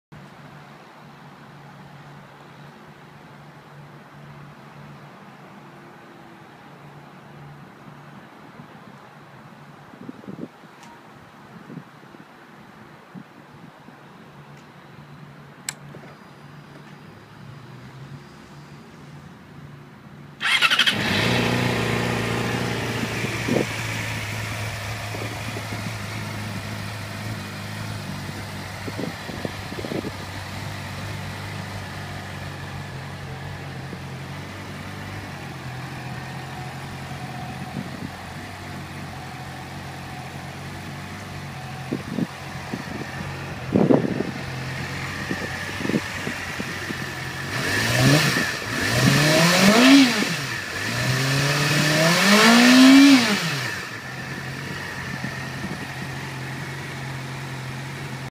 2010 Yamaha YZF-R6 at Motorangutan - Austin's Best place for Used Motorcycles
Austin used motorcycles for sale at Pink Gorilla Cycles.  This is a clean 2010 Yamaha YZF-R6.